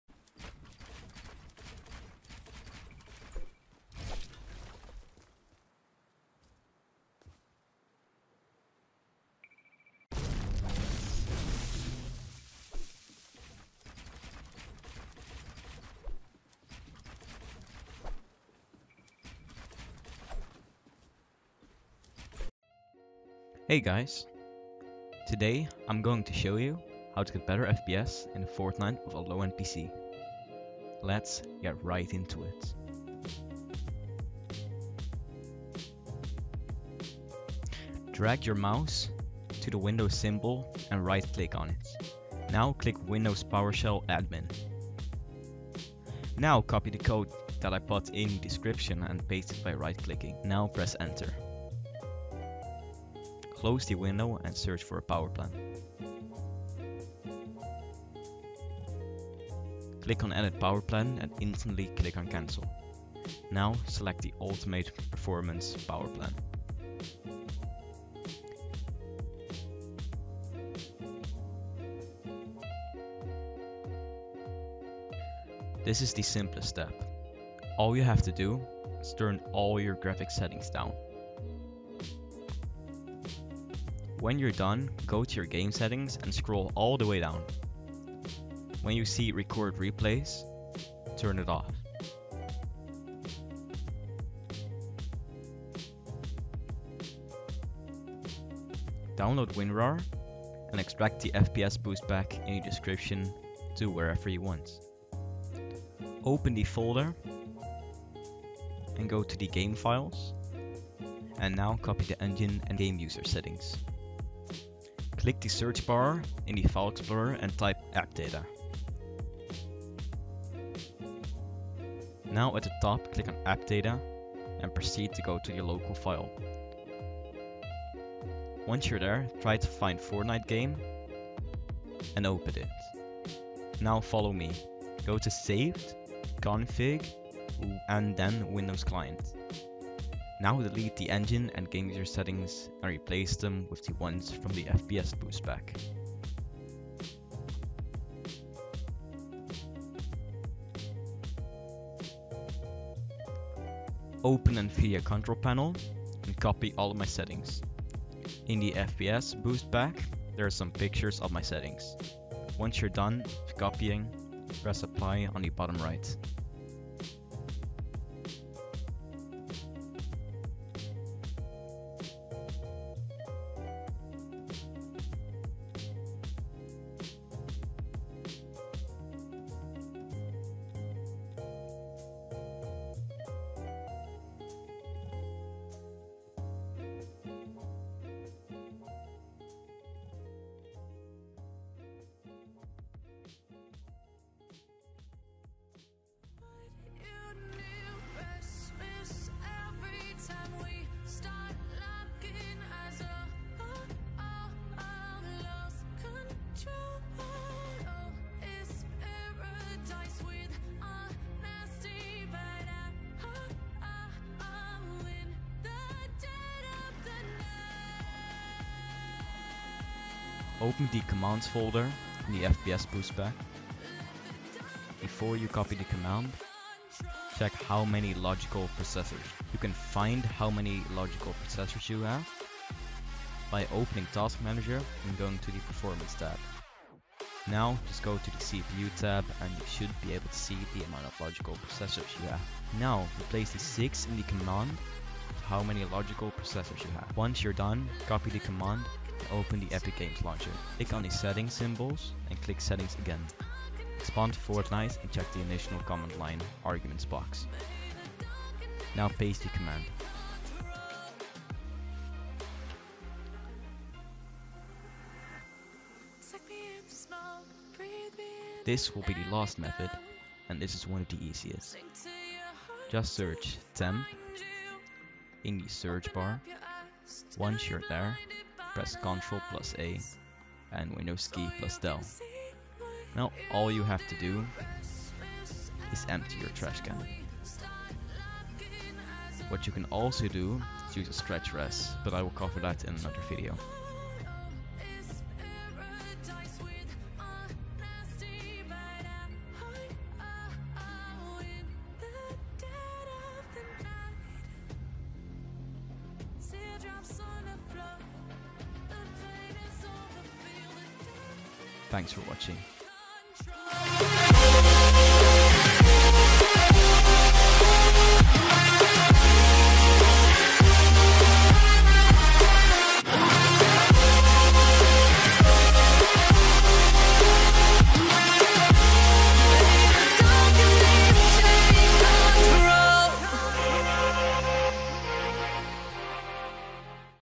FORTNITE ULTIMATE FPS BOOST SEASON 6 GUIDE FOR LOW END PC'S {STILL WORKING}
*Ultimate Powerplan Command:*

Script:

Hey guys, today i'm going to show you how to get better fps in fortnite with a low end pc, lets get right into it.

1:
Drag your mouse to the windows symbol and right click on it. Now click windows powershell admin.
Now copy the code that I put in the description and paste it by right clicking. Now press enter.
Close the window and search for power plan. Click on Edit Power plan and instantly click on cancel. Now select the ultimate performance power plan.

2:
This is the simplest step, all you have to do is turn all your graphics settings down.
Now go to your game settings and scroll all the way down. Now turn record replays off.

3:
Download winrar and extract the fps boost pack in the description to wherever you want. Open the folder and go to game files, now copy the engine and gameusersettings.
Click the search bar in file explorer and type %appdata%, now at the top click on appdata, and proceed to go to the local file. Once your there try to find FortniteGame.
Now delete the engine and gameusersettings and replace them with the ones from the fps boost pack.

4:
Open Nvidia Control Panel and copy all of my settings. In the fps boost pack there are some pictures of my settings.
Once ur done with copying press Apply on the bottom right.

5:
Open the Commands folder in the fps boost pack. Before you copy the command, check how many logicalprocessors you have. You can find how many logicalprocessors you have
by opening task manager and going to the performance tab. Now just go to the CPU tab and you should be able to see the amount of logicalprocessors you have.
Now replace the 6 in the command with how many logicalproccesors you have. Once ur done, copy the command and open the epic games launcher. Click on the settings
symbol and click settings again. Expand FORTNITE and check the additional command line arguments box. Now paste the command.

6:
This will be the last method and this is one of the easiest. Just search %temp% in the search bar. Once ur there press Ctrl + A and Windows key + Del. Now all you have
to do is empty your trash can.

You can also use a stretched res but I will cover that in another video.

Credit: Team Nova